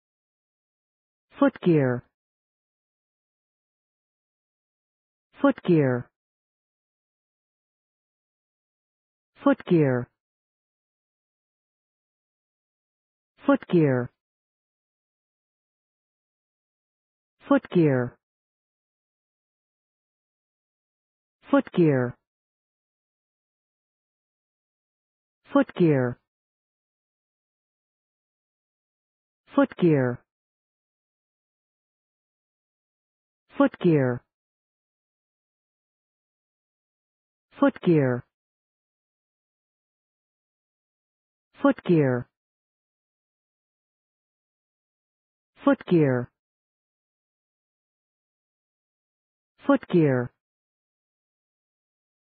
Pronunciation of Footgear | Definition of Footgear
Footgear pronunciation | How to pronounce Footgear in English?
/`fʊt,giːr/
Meaning of Footgear | What is Footgear?
(noun) covering for a person`s feet
What is Footgear | Footgear meaning and definition?